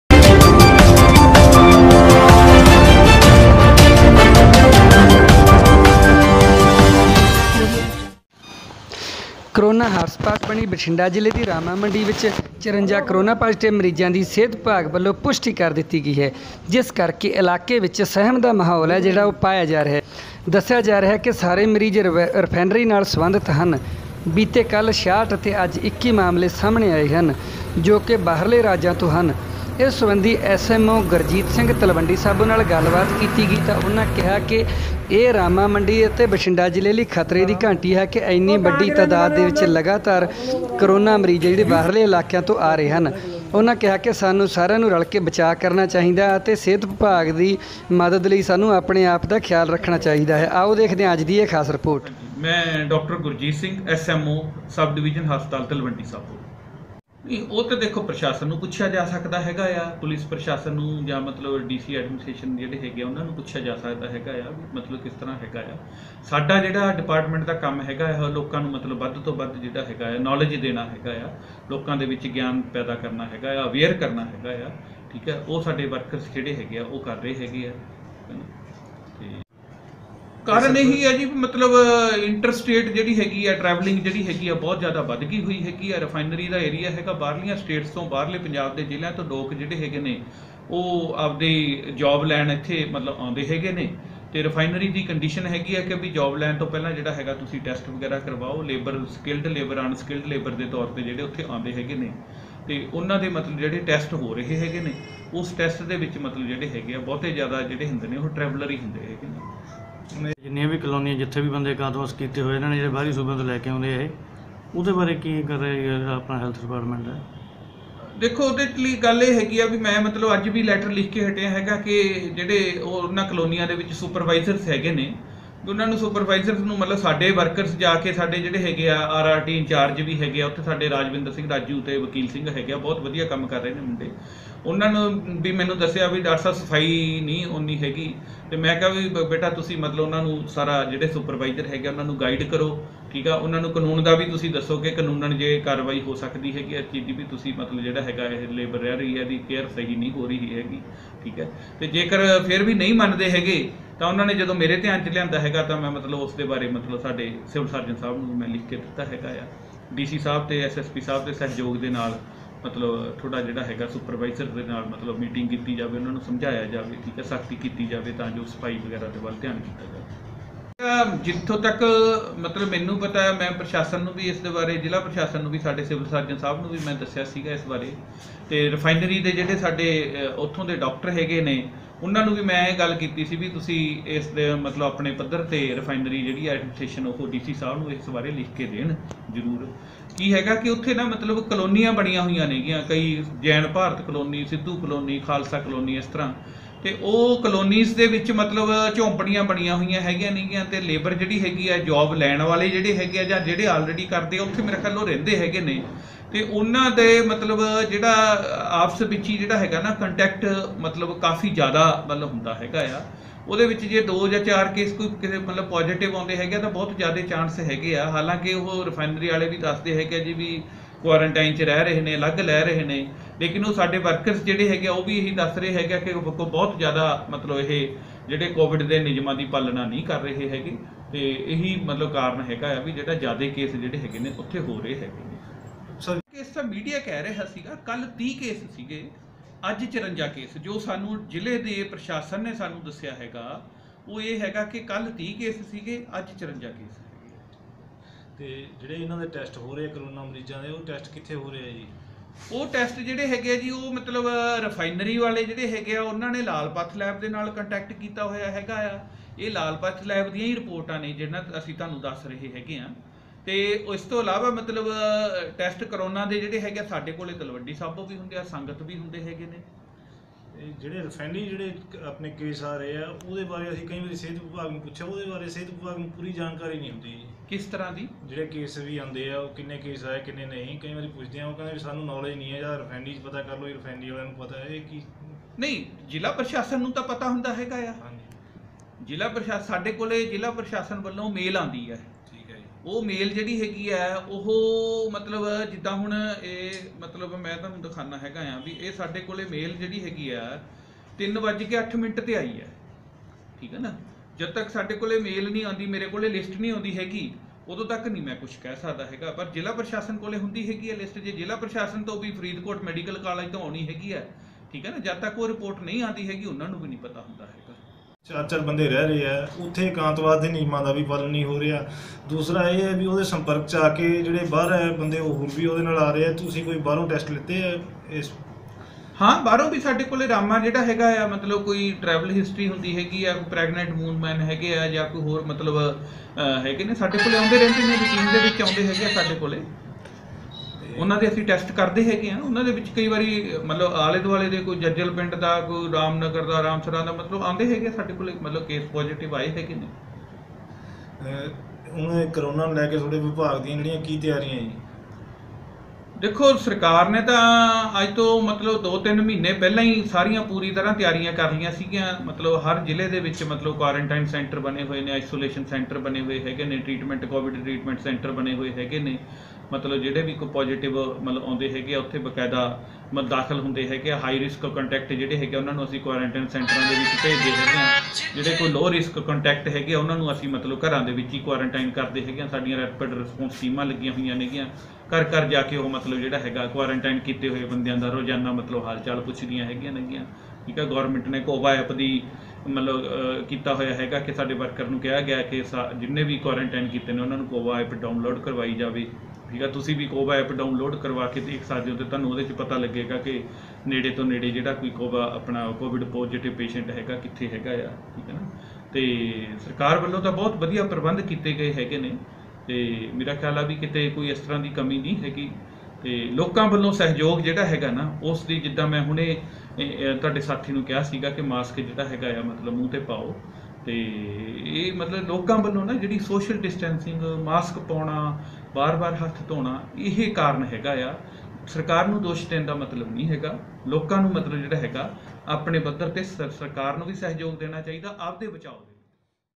ਰਾਮਾਂ ਮੰਡੀ ਰਿਫਾਇਨਰੀ ਵਿੱਚ 54 ਮਾਮਲੇ ਕਰੋਨਾ postive ਆਉਣ ਕਾਰਨ ਮੱਚਿਆ ਹੜਕੰਪ।
ਰਾਮਾਂ ਮੰਡੀ ਤੋਂ ਪੱਤਰਕਾਰ ਰਾਮਪਾਲ ਅਰੋੜਾ ਦੀ ਰਿਪੋਰਟ।